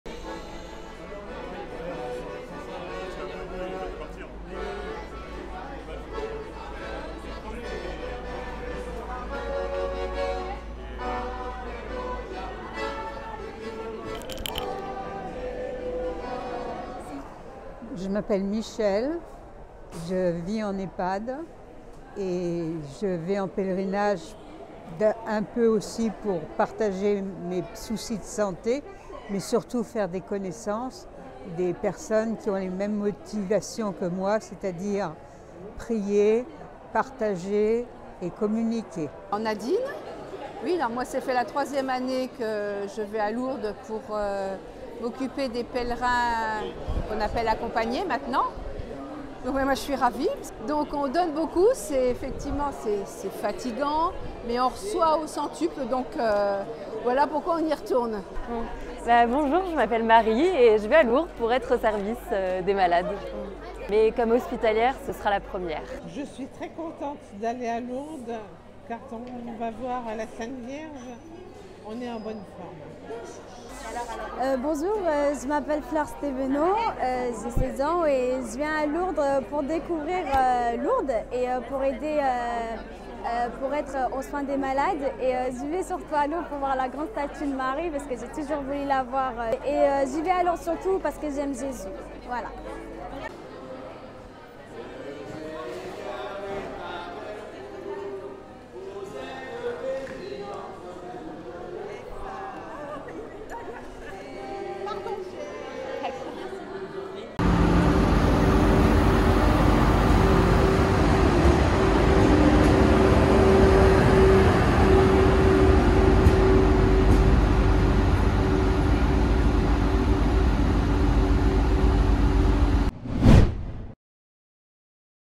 Tous pèlerins à Lourdes
Aujourd'hui, plus de 500 pèlerins prenaient le chemin vers Lourdes jusqu'au 22 août. Répondant tous à l'appel de Marie, ils partent pour partager la joie, les prières et des moments de paix tous ensemble !